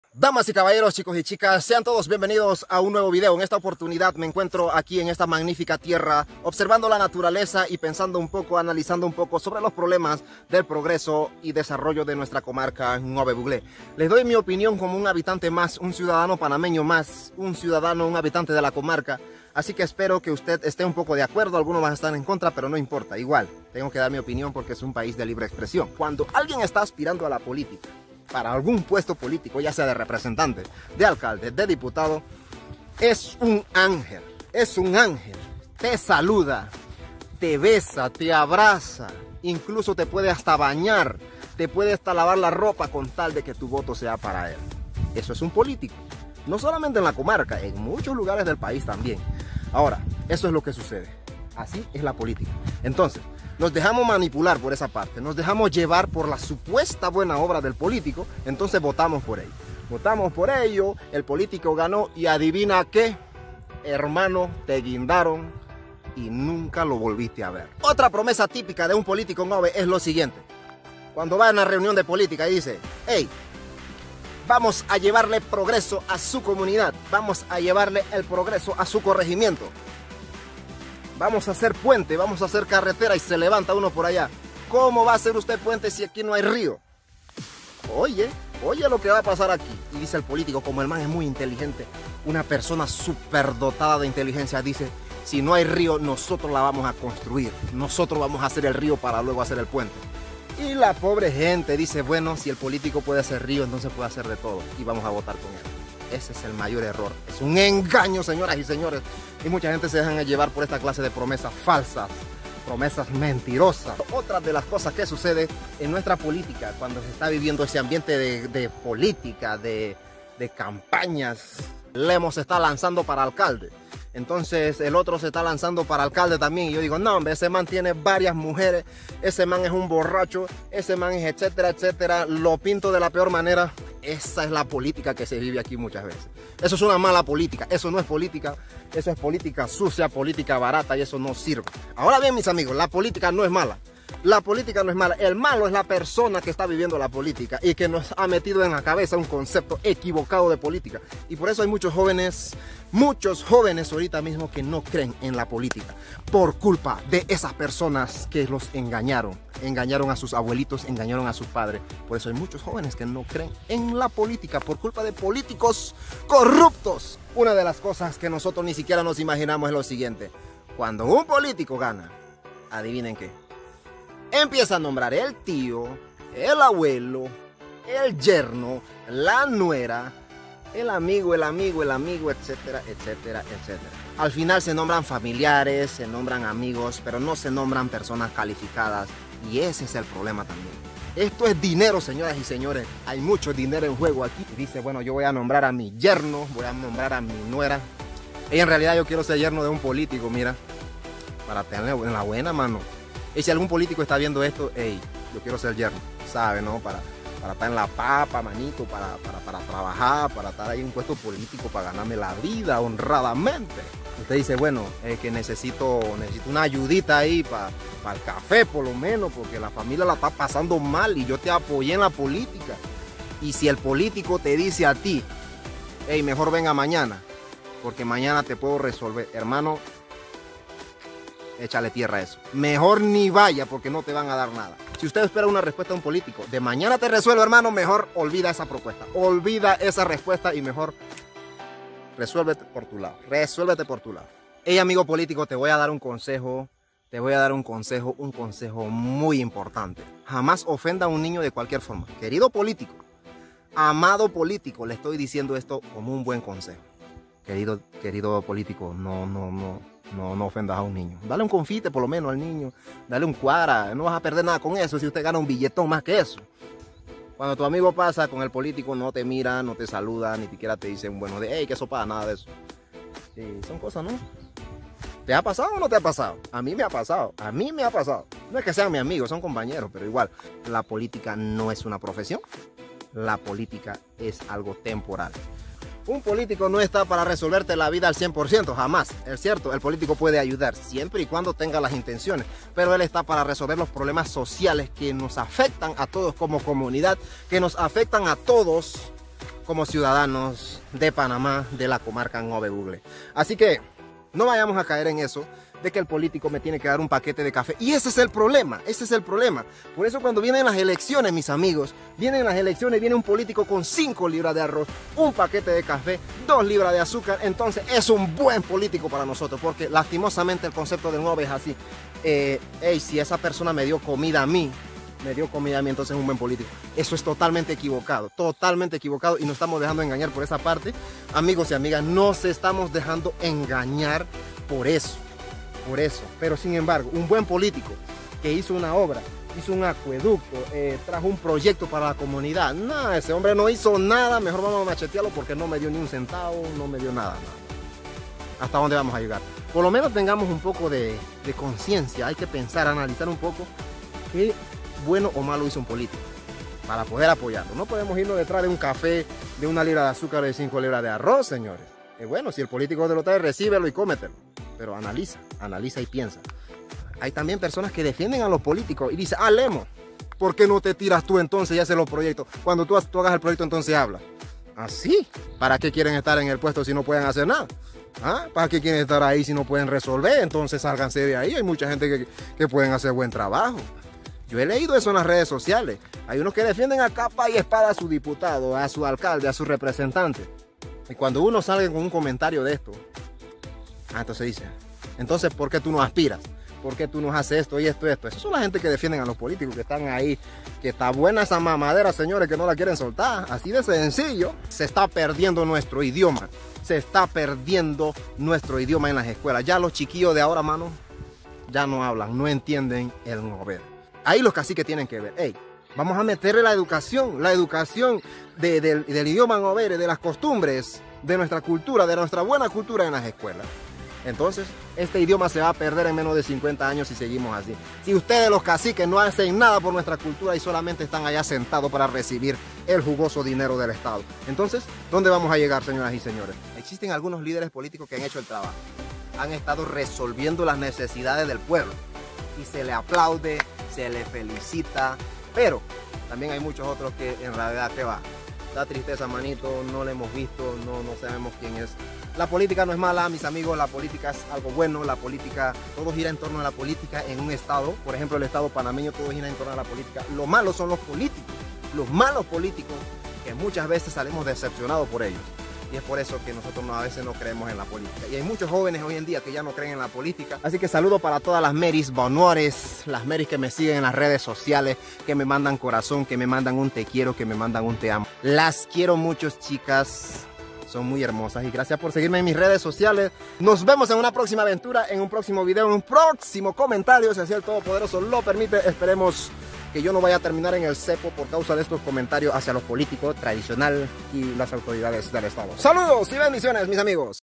La realidad de la política en la Comarca Ngäbe Buglé
Un análisis como un habitante de la comarca y como ciudadano de Panamá. Las realidades que se viven en cuanto al tema político.